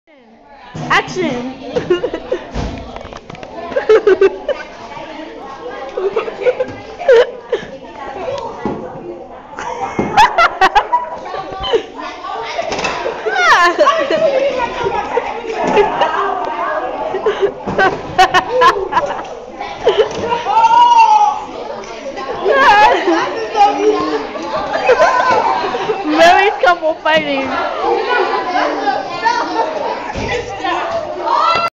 myasia & uchenna figting full version
a vid we made last year in 6th grade where uchenna beats up myasia cause of a funny argument durinq ELA class. They used to fight all the time. We're all BFF though [: nothing major, just goofing off. A little taste of how bad we were last year. if you hear closely, you can hear out teacher yelling in the backround [: PS there was no students hurt, and only 1 strand of weave pulled out in the making of this video [: